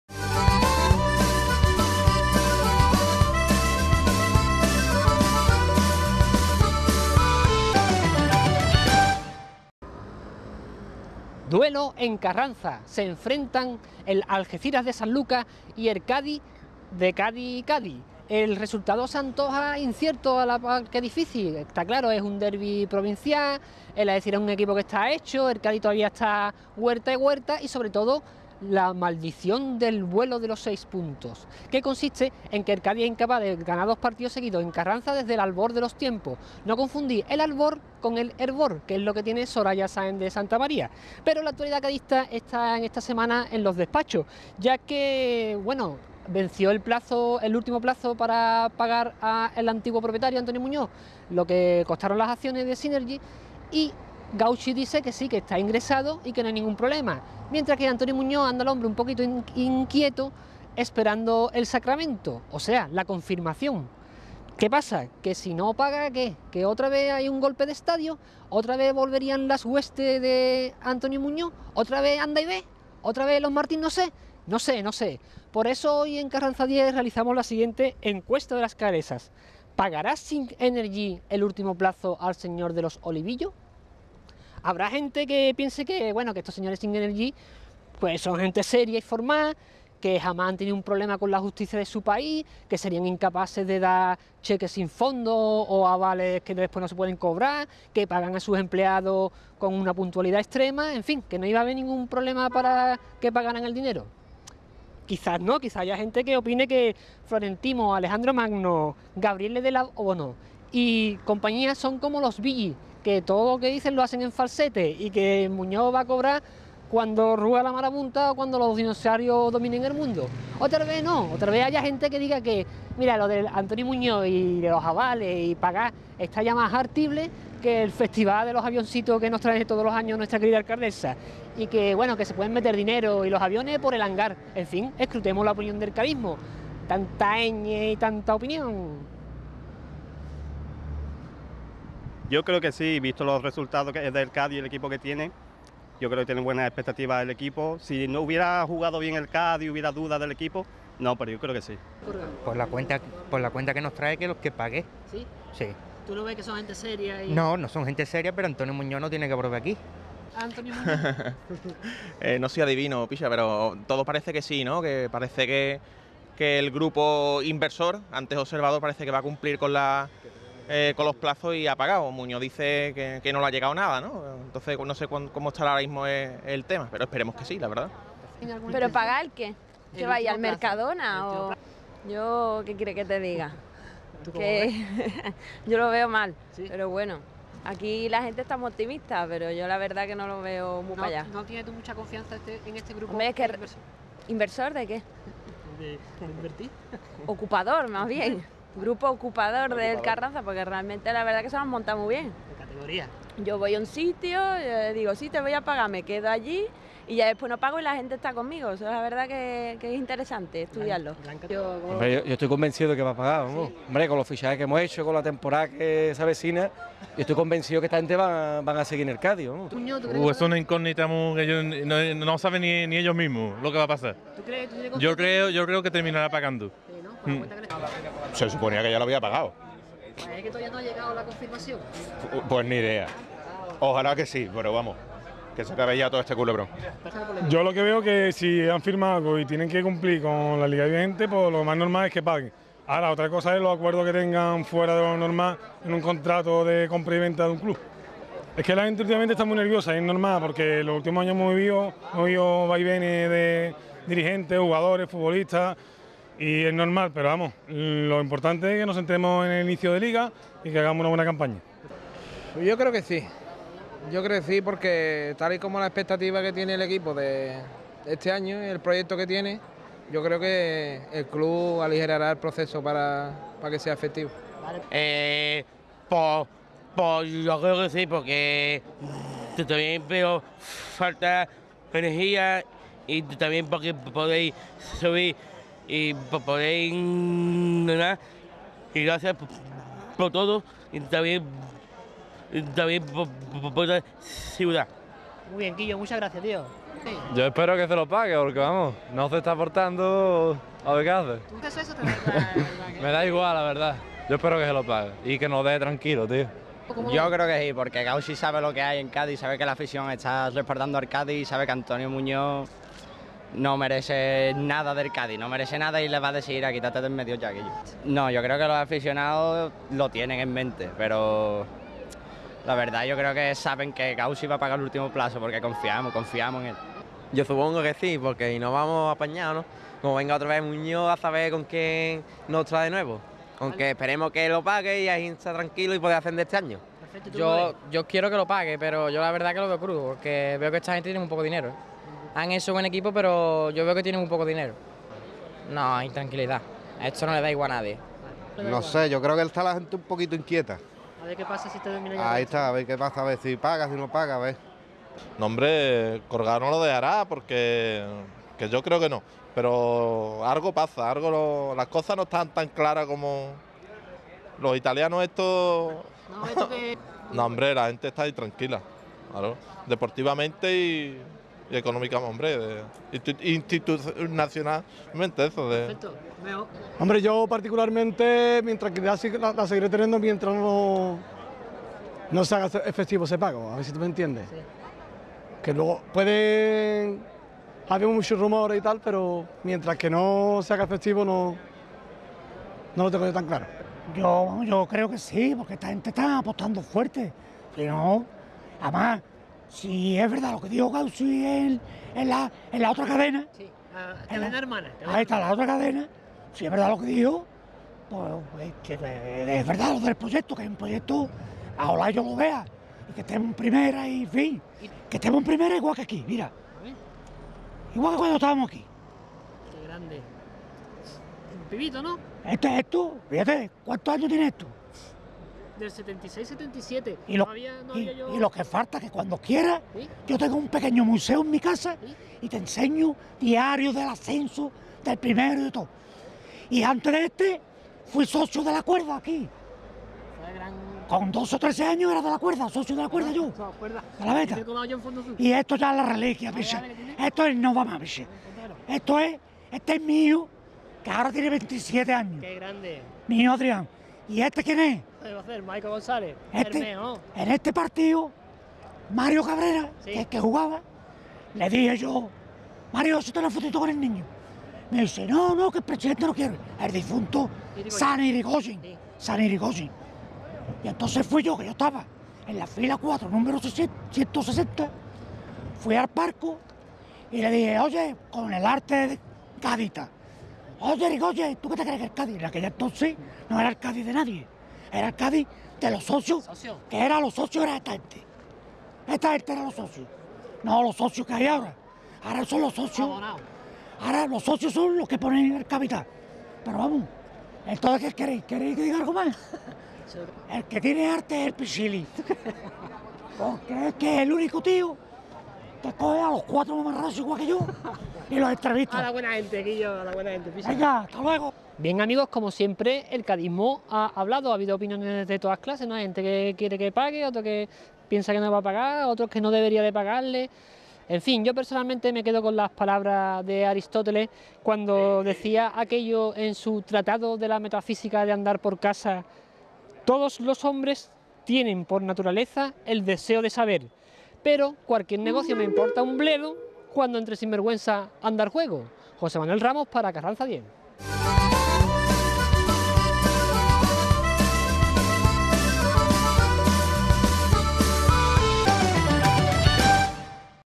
Encuesta de las Calesas (09-09-13)
José Manuel Ramos sale a la calle en la previa de los partidos del Cádiz para consultar a los aficionados sobre tema de actualidad del conjunto cadista.